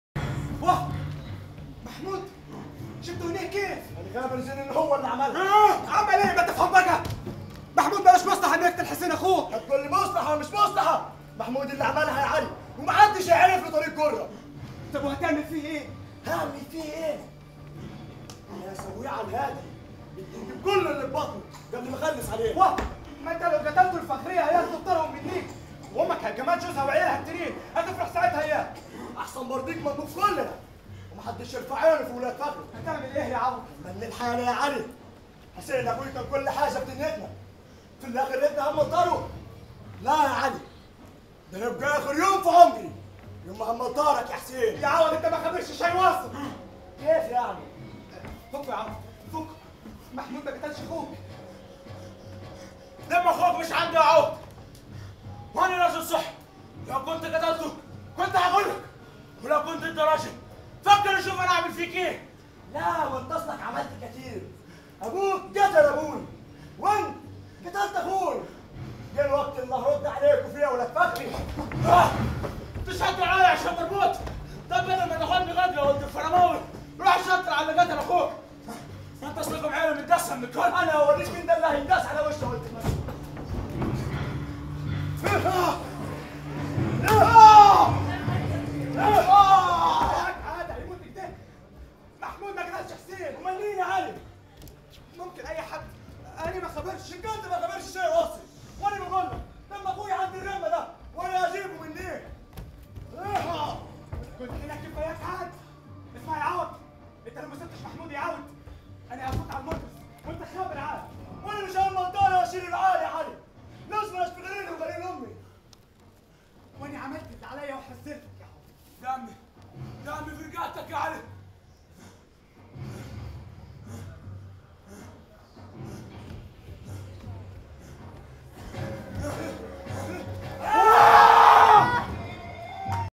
مشهد أبدع فيه طلاب كلية الإعلام.. في مسرحية "قتل خطأ
" مسرح_الجامعة " عبارة عن مجموعة من العروض المسرحية لطلاب الجامعات بشكل عام وجامعة القاهرة بشكل خاص، ويستعرض الطلاب مواهبهم الإبداعية والتمثيلية في عروض ترتقي إلى درجة الاحتراف.